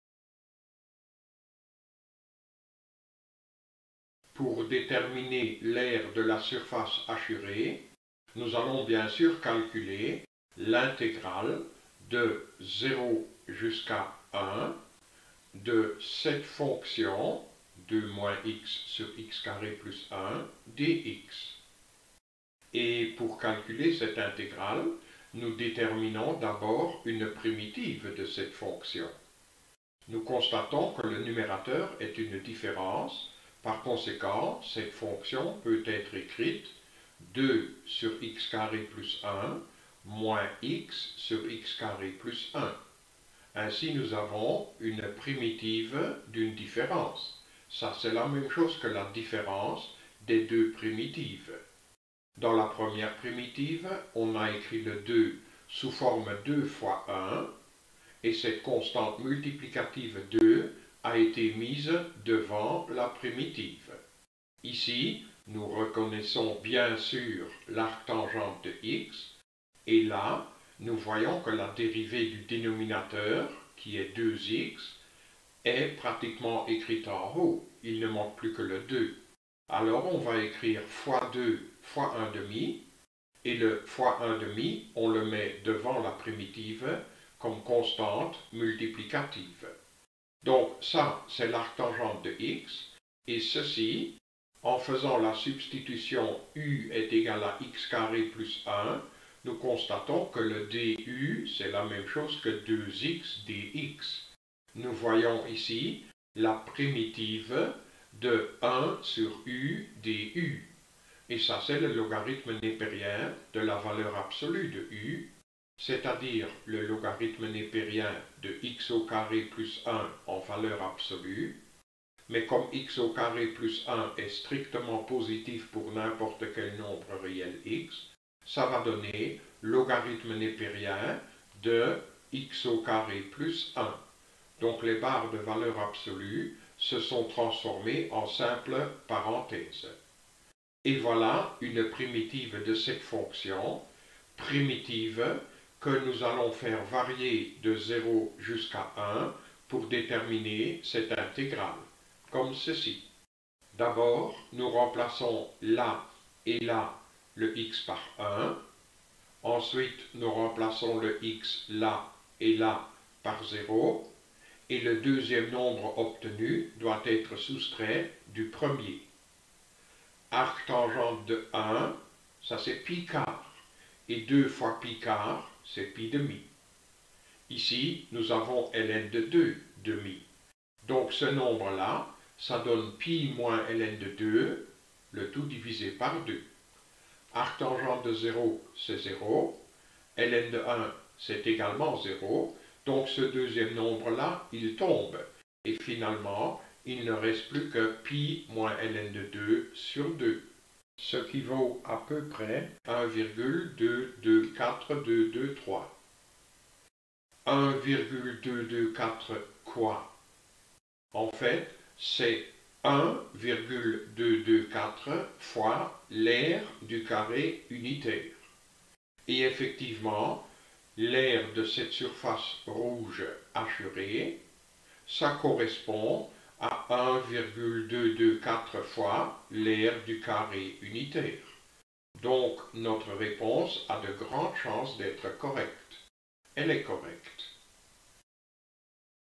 Intégrales 003
Nous calculons ici une aire par une intégrale.